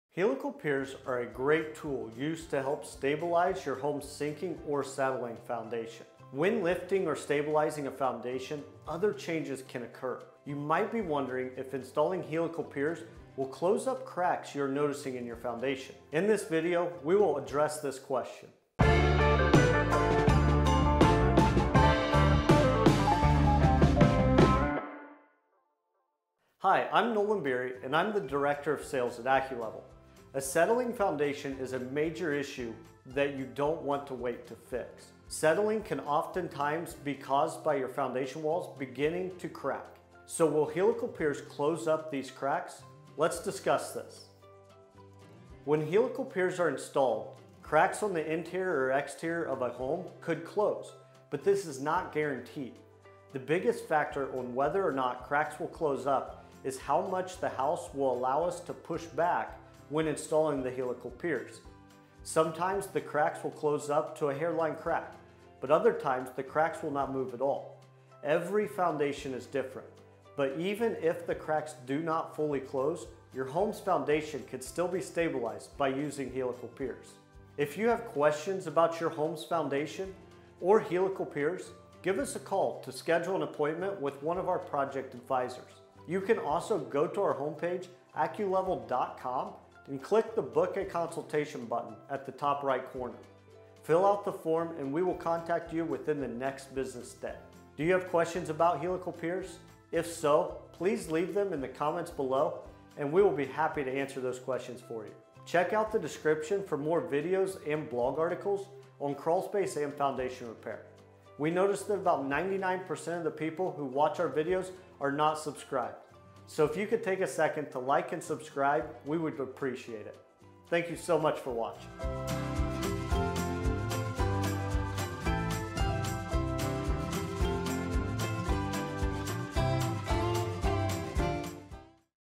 Do Helical Piers Close Up Cracks?
Is your home's foundation settling, or are you noticing cracks in your foundation? Helical piers might be the best option for you, but do they close up those cracks? In this video we discuss just that!

A helical pier is a steel shaft with screw-like plates on the end. This pier is screwed through the ground outside of your home until it hits stable, undisturbed soil. The pier has a steel bracket that is positioned under your foundation’s footing, much like the seat of a chair, to give the wall support and hold it in place.  This bracket allows the pier to leverage the resistance between the deep solid ground at the bottom of the pier and your home.

TIMESTAMPS
►0:00 Introduction
►0:44 Will Helical Piers Close Up Cracks?
►1:15 Outro